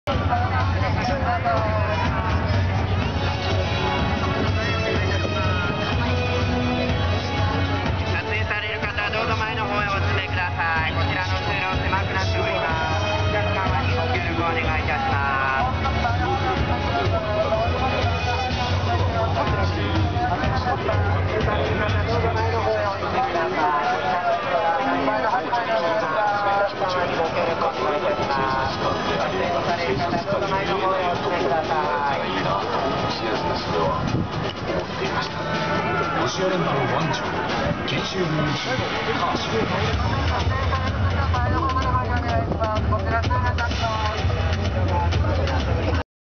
98式イングラム 幕張に立つ
映画の宣伝でパトレイバー　98式イングラムが幕張のQＶCマリン
フィールドにやってきました。撮影は平成26年4月20日です。